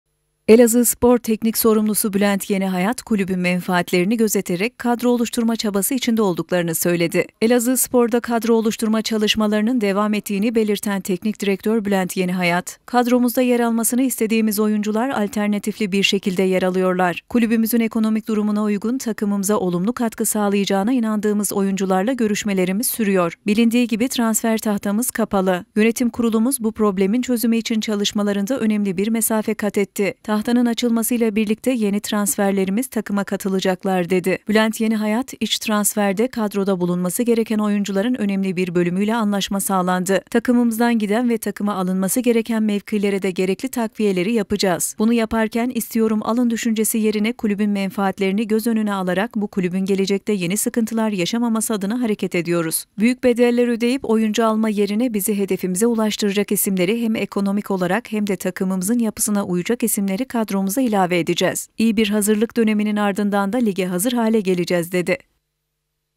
YENİHAYAT, KULÜBÜN MENFAATLERİNİ GÖZETEREK KADRO OLUŞTURMA ÇABASI İÇERİSİNDE OLDUKLARINI SÖYLEDİ
ELAZIĞSPOR TEKNİK SORUMLUSU BÜLENT YENİHAYAT, KULÜBÜN MENFAATLERİNİ GÖZETEREK KADRO OLUŞTURMA ÇABASI İÇERİSİNDE OLDUKLARINI SÖYLEDİ